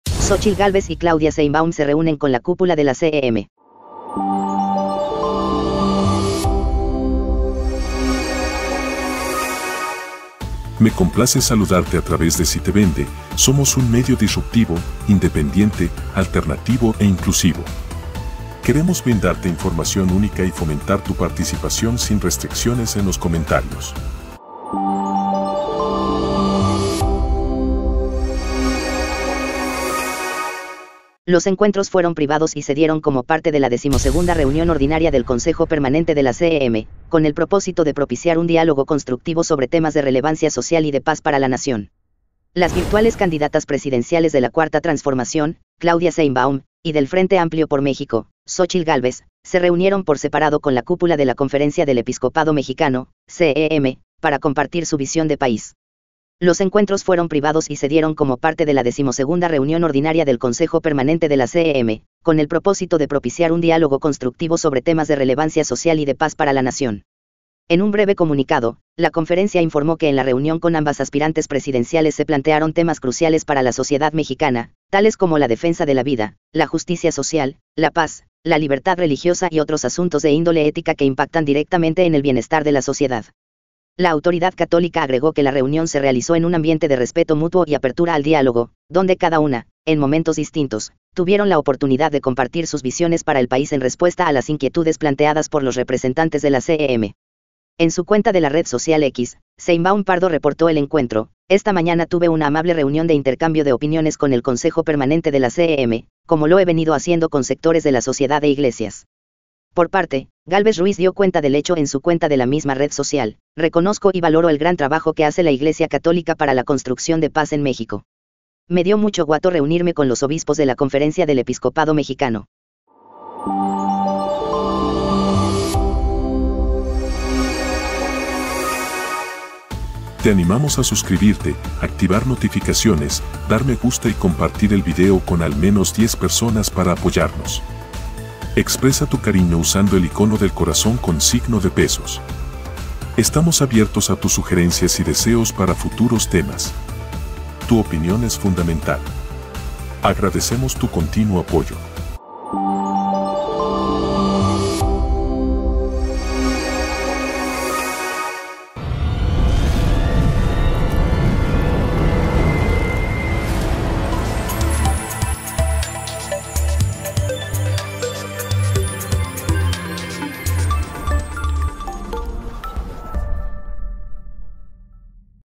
Xóchitl Gálvez y Claudia Sheinbaum se reúnen con la cúpula de la CEM
• Xóchitl Gálvez y Claudia Sheinbaum se reúnen con la cúpula de la CEM. Los encuentros fueron privados y se dieron como parte de la XII Reunión Ordinaria del Consejo Permanente de la CEM, con el propósito de “propiciar un diálogo constructivo sobre temas de relevancia social y de paz para la nación”.. Las virtuales candidatas presidenciales de la Cuarta Transformación, Claudia Sheinbaum, y del Frente Amplio por México, Xóchitl Gálvez, se reunieron por separado con la cúpula de la Conferencia del Episcopado Mexicano (CEM) para compartir su visión de país.. Los encuentros fueron privados y se dieron como parte de la XII Reunión Ordinaria del Consejo Permanente de la CEM, con el propósito de “propiciar un diálogo constructivo sobre temas de relevancia social y de paz para la nación”.. En un breve comunicado, la Conferencia informó que en la reunión con ambas aspirantes presidenciales “se plantearon temas cruciales para la sociedad mexicana, tales como la defensa de la vida, la justicia social, la paz, la libertad religiosa y otros asuntos de índole ética que impactan directamente en el bienestar de la sociedad”. La autoridad católica agregó que la reunión se realizó “en un ambiente de respeto mutuo y apertura al diálogo, donde cada una, en momentos distintos, tuvieron la oportunidad de compartir sus visiones para el país en respuesta a las inquietudes planteadas por los representantes de la CEM”.. En su cuenta de la red social X, Sheinbaum Pardo reportó el encuentro: “Esta mañana tuve una amable reunión de intercambio de opiniones con el Consejo Permanente de la CEM, como lo he venido haciendo con sectores de la sociedad e iglesias”.. Por parte, Gálvez Ruiz dio cuenta del hecho en su cuenta de la misma red social: “Reconozco y valoro el gran trabajo que hace la iglesia católica para la construcción de paz en México. Me dio mucho guato reunirme con los obispos de la Conferencia del Episcopado Mexicano”.